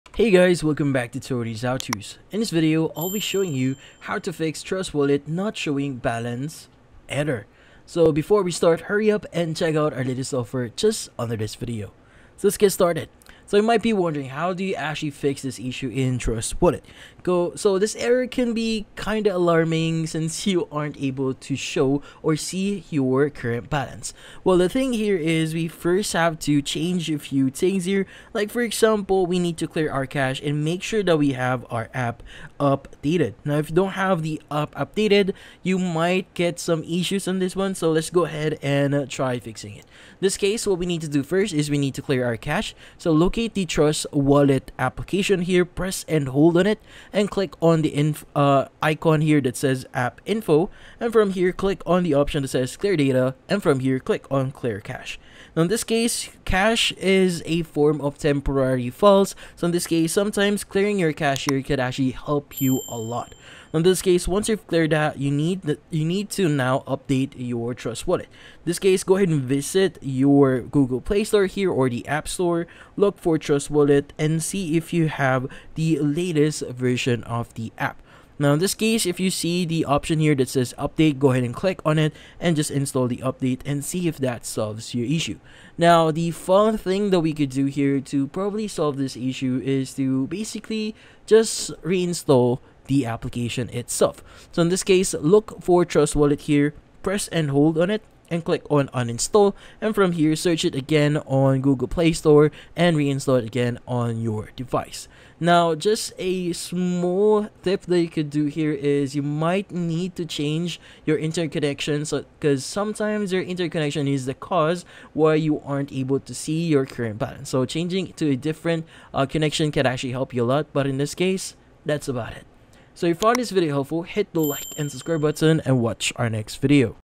How to Fix Trust Wallet Not Showing Balance (Error Fixed, 2026)
In this video I show you How to Fix Trust Wallet Not Showing Balance (Error Fixed) in 2026. The quick and easy tutorial how to solve trust wallet error balance not visible. Do you want to know step by step cannpt check the balance in trust wallet in 2026? Yes? Great!